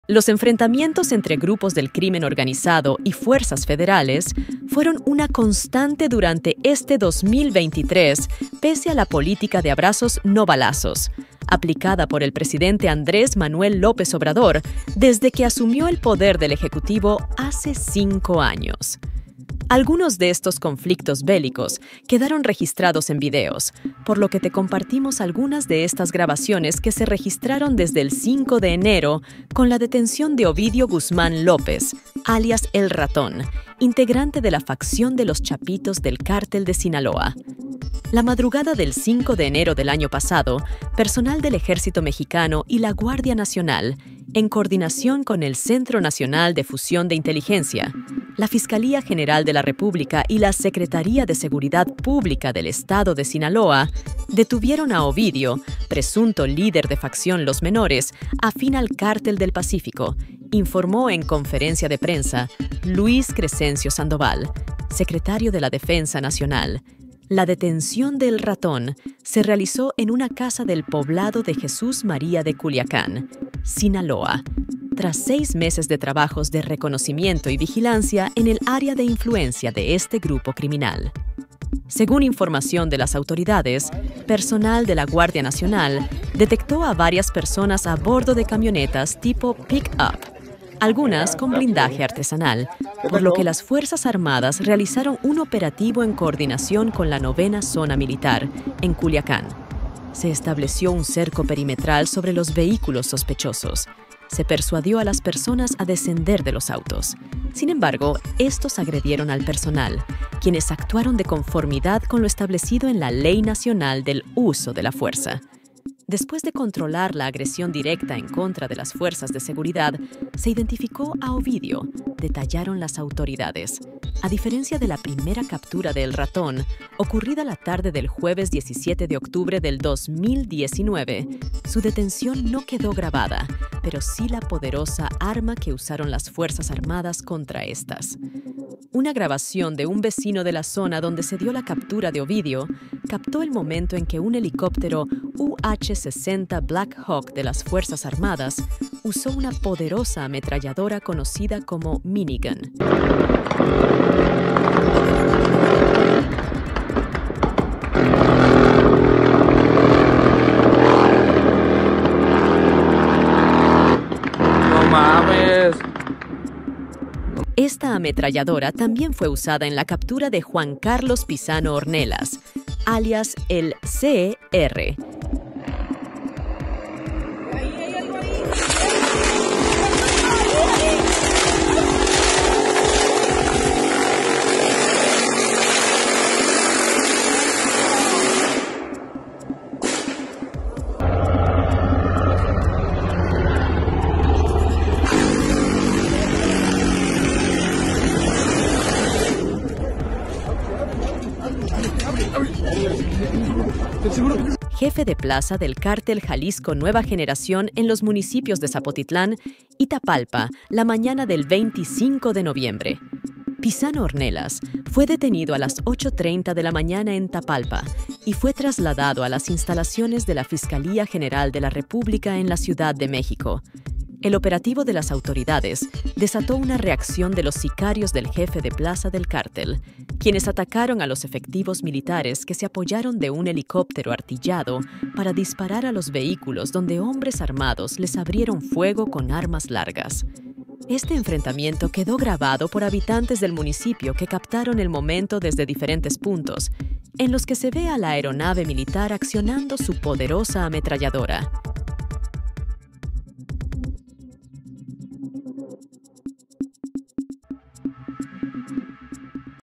LOS CHAPITOS y CJNG VS EJÉRCITO MEXICANO
En este video descubrirás la verdad detrás del conflicto entre los Chapitos, CJNG y el Ejército Mexicano. Mira el impactante enfrentamiento y entérate de cómo AMLO y las fuerzas armadas están involucrados en esta guerra.

No te pierdas esta información exclusiva de Vortice Noticias sobre la situación actual en México y cómo podría afectar a las elecciones presidenciales de 2024. Acompáñanos y descubre la realidad detrás de los titulares.

LOS CHAPITOS y CJNG VS FUERZAS ARMADAS, DOS ENFRENTAMIENTOS ‘DE PELÍCULA’ EN EL 2023.